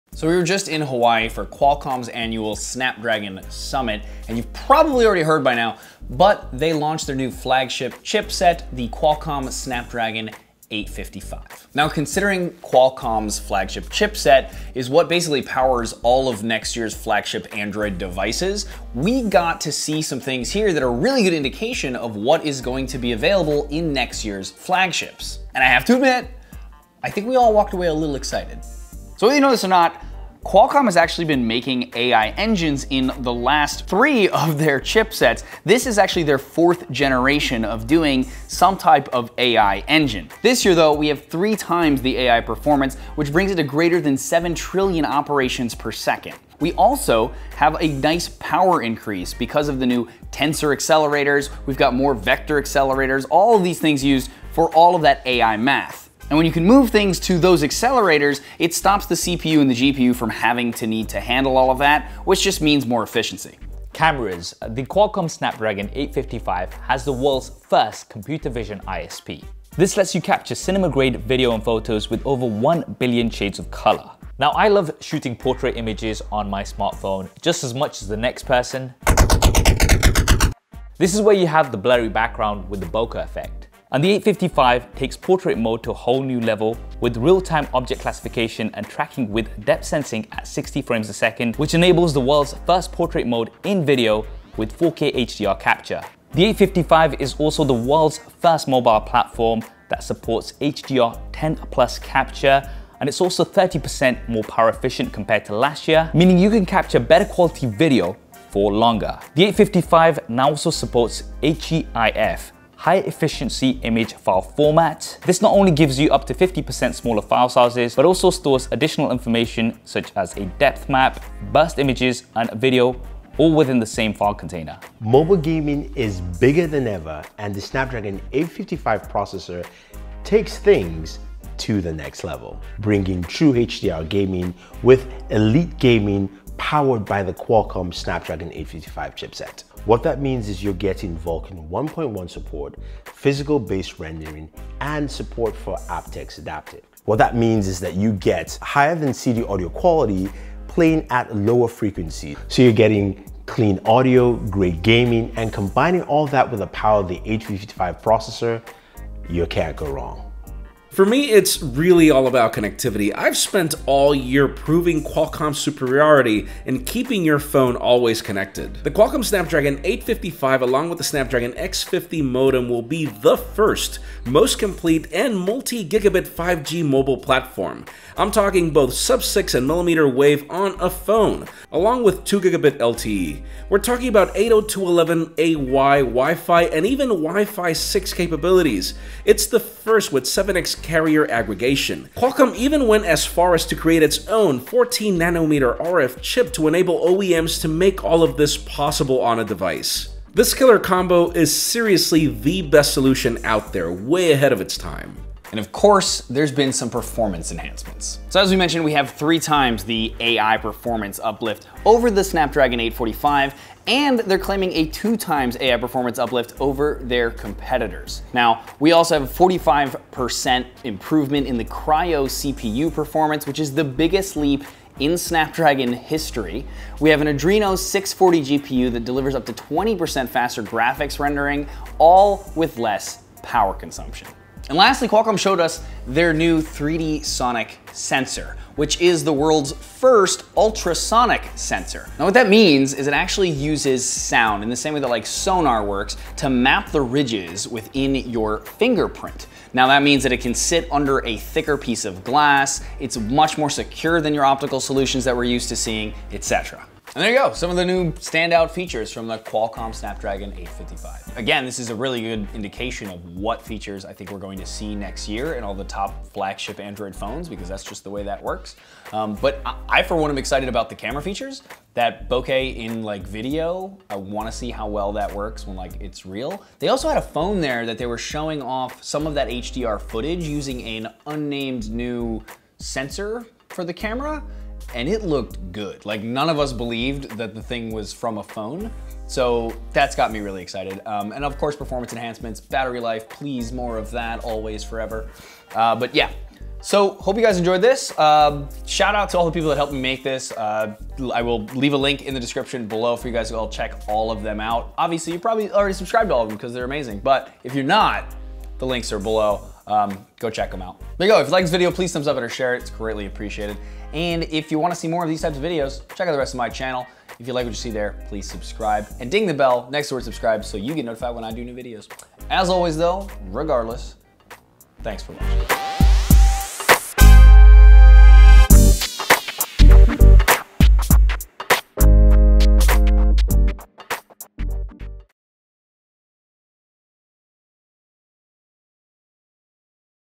Why You Should Be Excited About the Qualcomm Snapdragon 855
Qualcomm showed us the latest flagship chipset in Hawaii recently. Heere is why you should be excited about the new Qualcomm Snapdragon 855.

Thanks to Qualcomm for partnering with me to do this video!